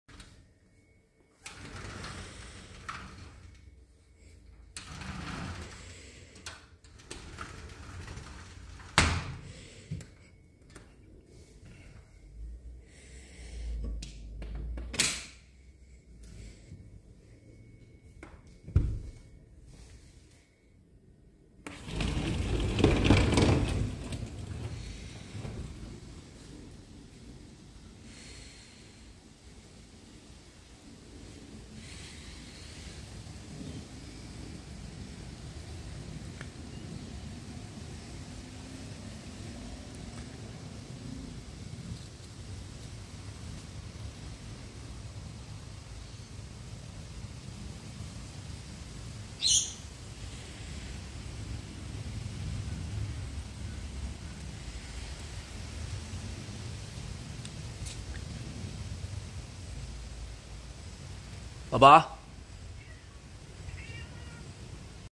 1minutecatvideo
Watch as My Cute Cat explores their surroundings and plays with their favorite toys in this adorable cat video! From chasing laser pointers to snuggling up on the couch, My Cute Cat is sure to bring a smile to your face with their playful antics. Don't forget to give them some love and a few treats after watching!